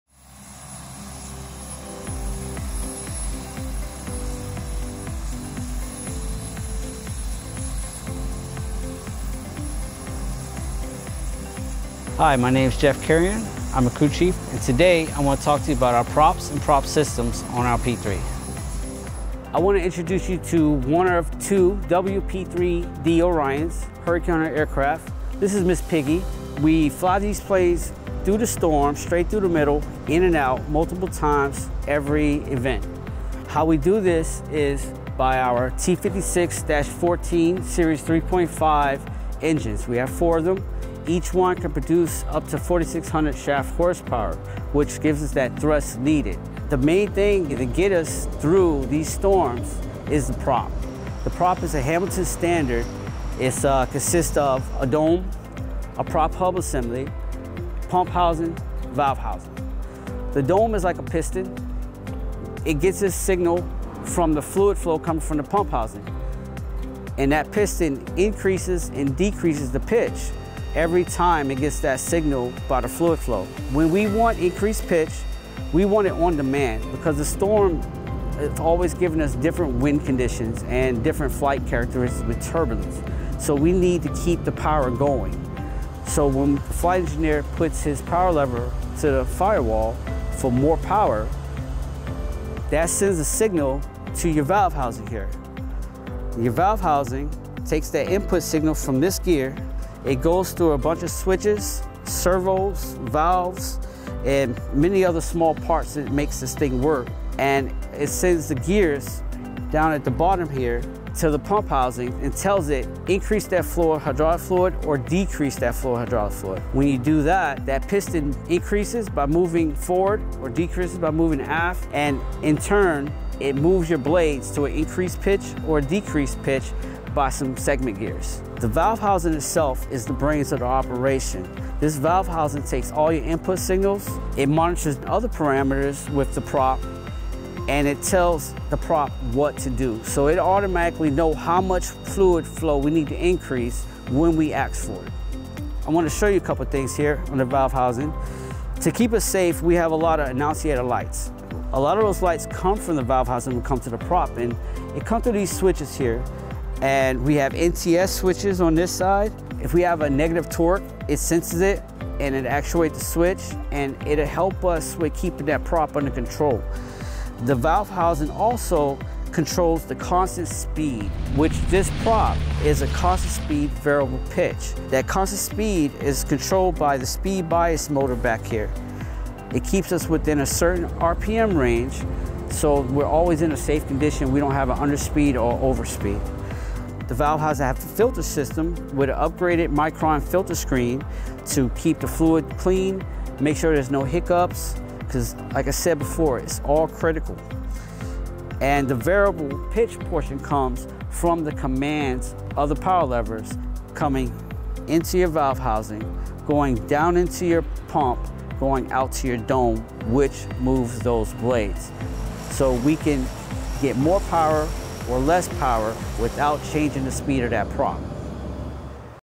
Maintenance Monday: The P-3 Propeller
Welcome to the first edition of our "Maintenance Monday" series! Where we'll periodically be bringing you along for a behind-the-scenes look at the measures we take to ensure the NOAA aircraft fleet keeps the wind under their wings for years to come. Look out for more to come!

First up: the propeller of our Lockheed WP-3D Orion